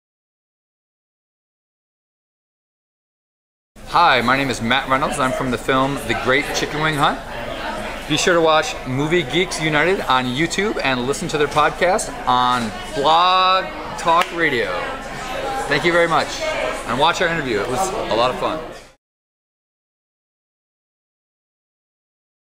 MGU @Atlfilm365 Coming Soon: Director Matt Reynolds
Atlanta Film Festival aka Atlfilm365 Matt Reynolds director of "The Great Chicken Wing Hunt" promoting Movie Geeks United. Recorded for MGU chicken wing enthusiasts. :-)

For more information on Movie Geeks United, and to access our archive of over 700 filmmaker interviews,

Co-Host:Dean Treadway
Camera Operator & Editor: Rich Gedney